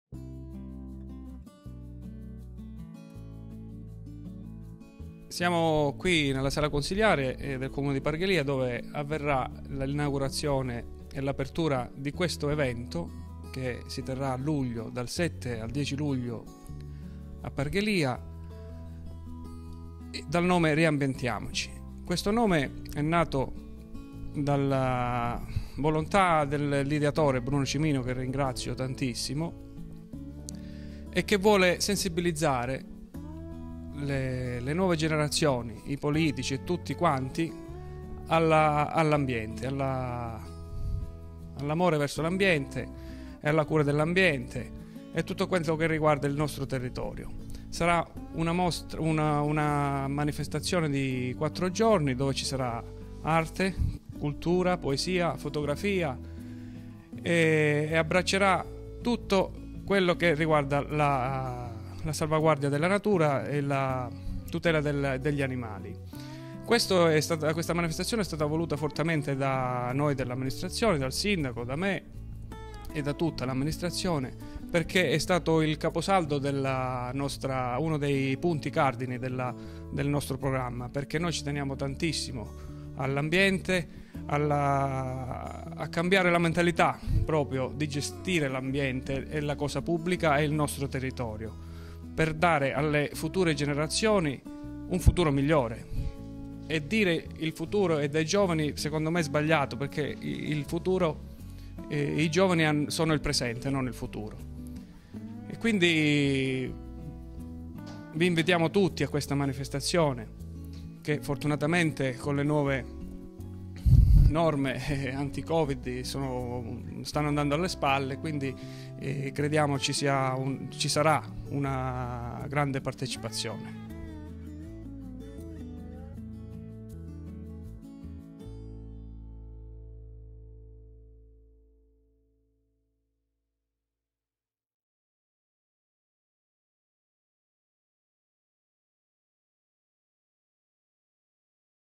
Tommaso Belvedere - Quattro giornate per tutelare il patrimonio naturale - Parghelia
Intervista a Bruno Belvedere, Vicesindaco e Ass. al Turismo di Parghelia (VV) - Quattro giornate per tutelare il patrimonio naturale - 16 aprile 2022 - A cura di Antonio Abbruzzese, Riccardo Cristiano e Gianni Colacione